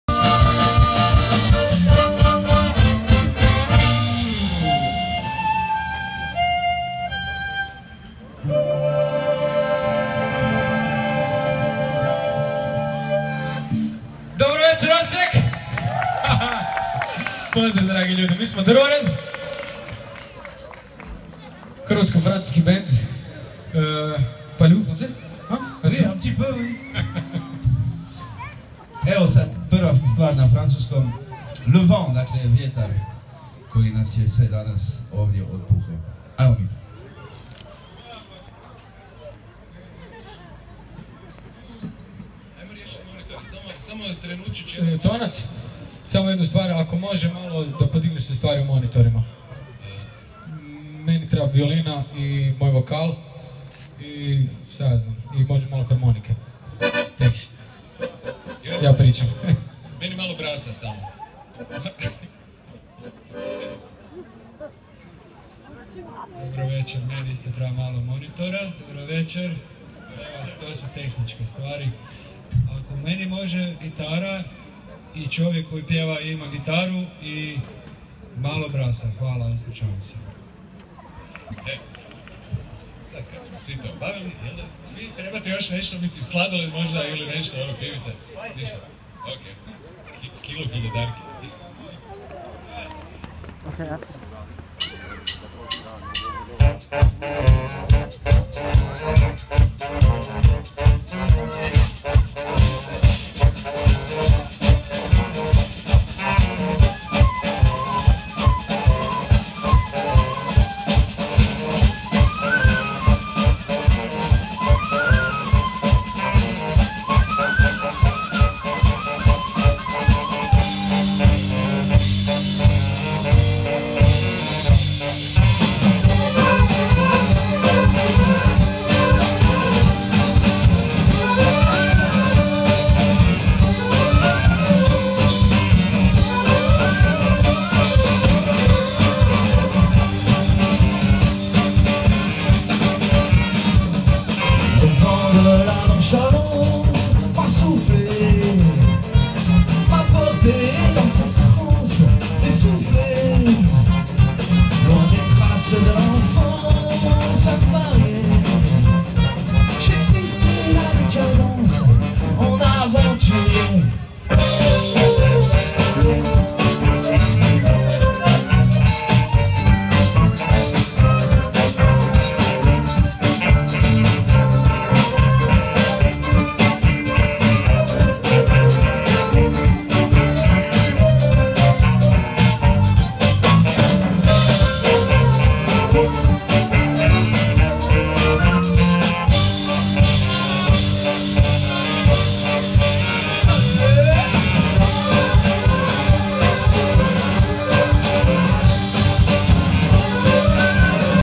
Osijek Jazz Music
A walk around Osijek and in the middle of the park a Croatian and French Jazz band playing, how cool is that.